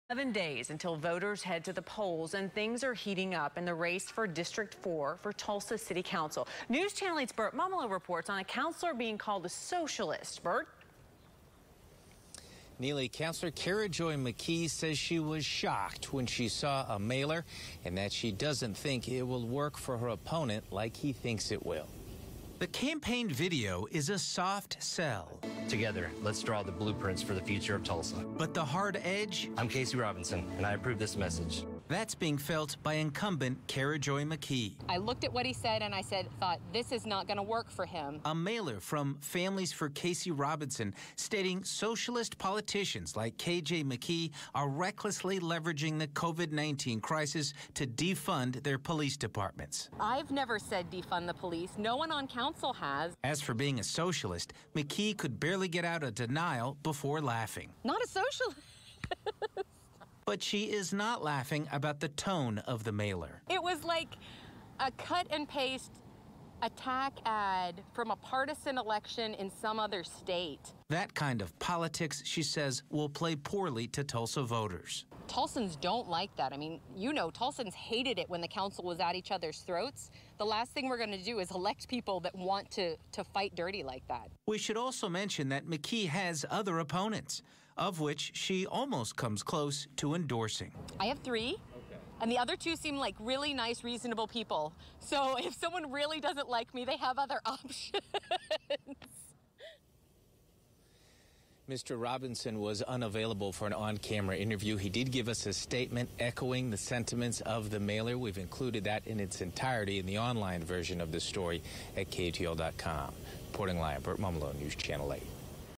Controversial campaign mailer shocks Tulsa city councilor
A Tulsa city councilor is speaking out after a campaign mailer caught her off guard. A mailer from Families for Casey Robinson stated, "Socialist politicians like KJ McKee are recklessly leveraging the Covid-19 crisis to defund their police departments."

NewsChannel 8's Burt Mummolo reports.